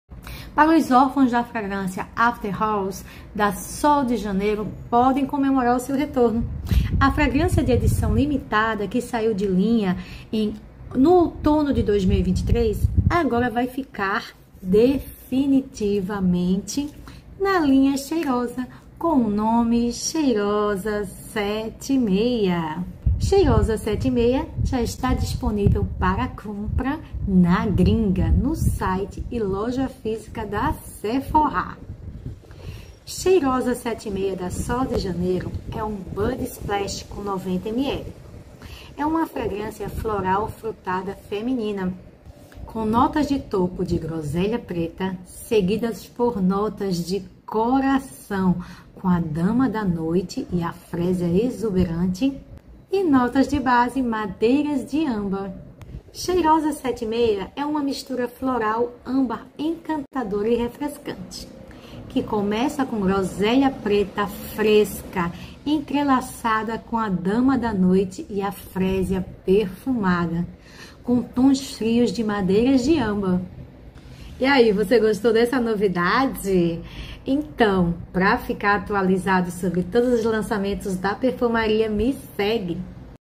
Lançamento Cheirosa 76 da Sol de Janeiro. Saiba de tudo!
Lançamento na Gringa.
Disponível para compra no site e lojas físicas da Europa do grupo Sephora.
Cheirosa 76 da Sol de Janeiro.